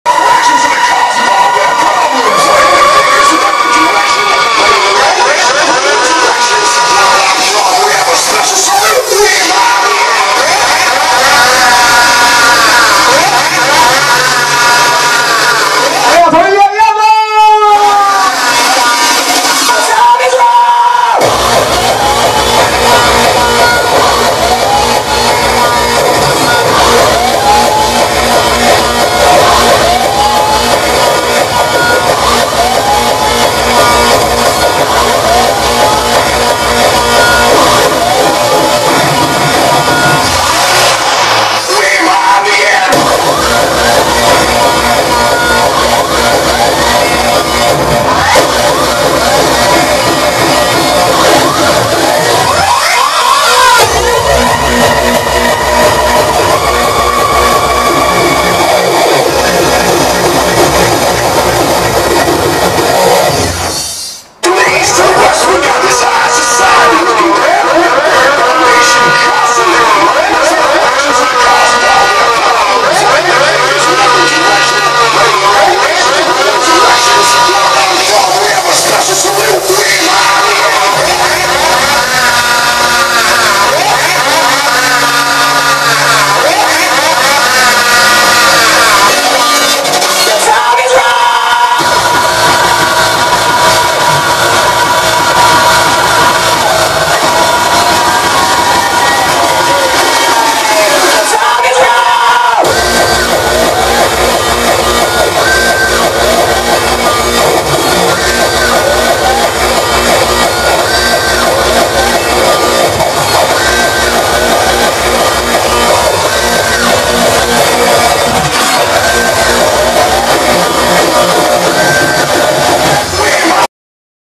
DJ Killscream Apocalypse Festival Eleven Vallgorguina 23/4/2016
un vídeo de la sesión de DJ Killscream del festival Apocalypse United By Hardcore Festival: 12 Horas Non Stop en la sala exterior de la discoteca Eleven de Vallgorguina